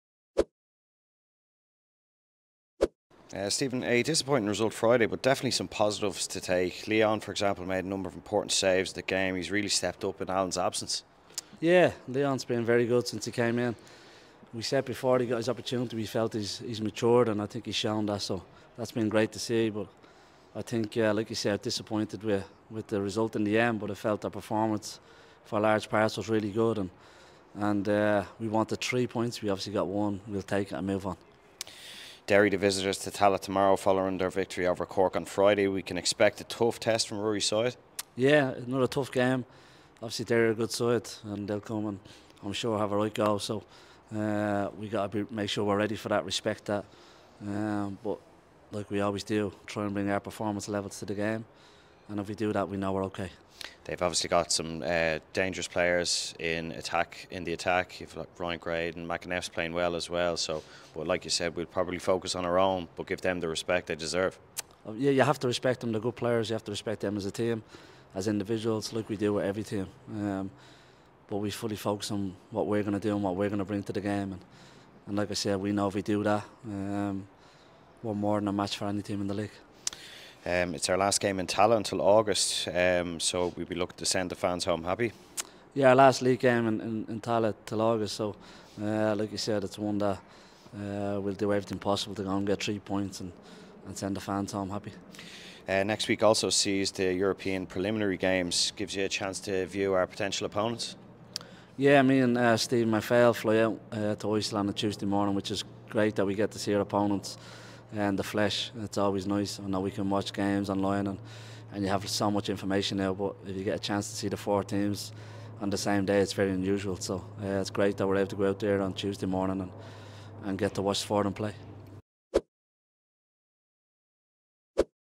Stephen Bradley l Pre Match Interview v Derry City l 25 June 2023
Stephen Bradley previews Monday's top of the table clash at Tallaght Stadium